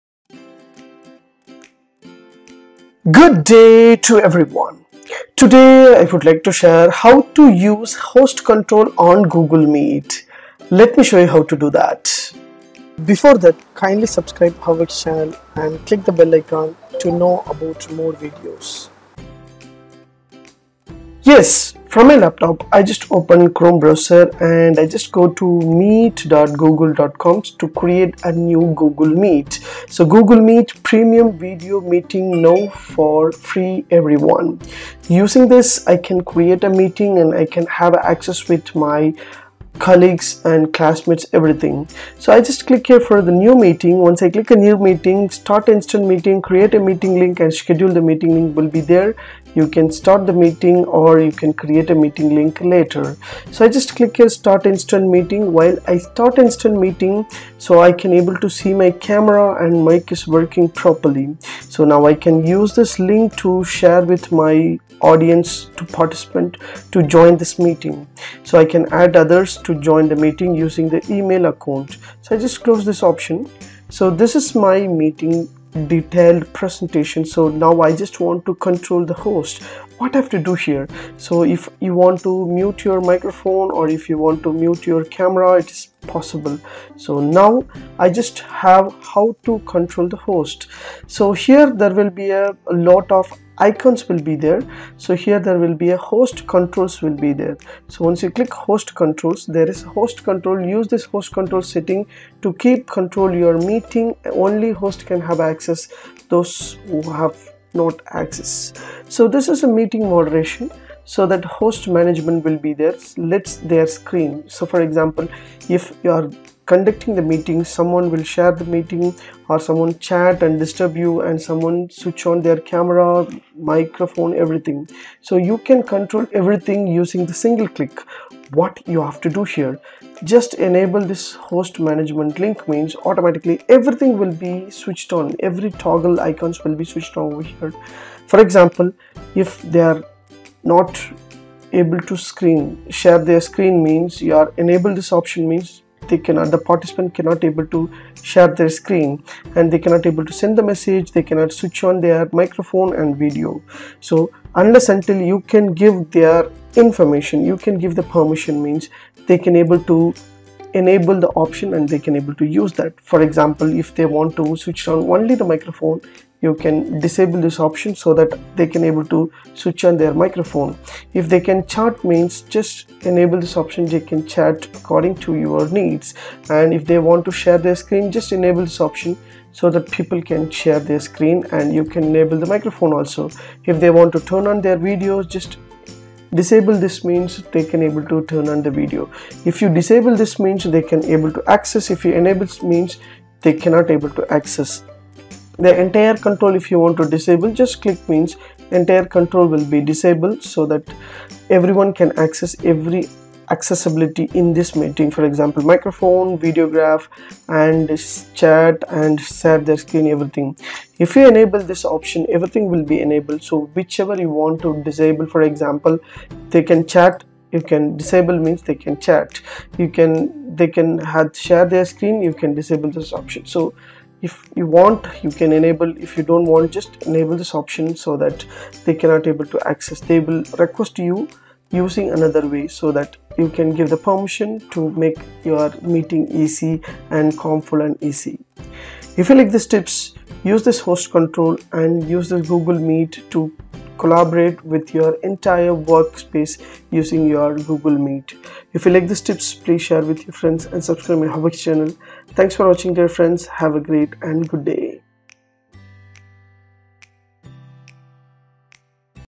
How to use host control on Google Meet
This video tutorial will help you  How to use host control on Google Meet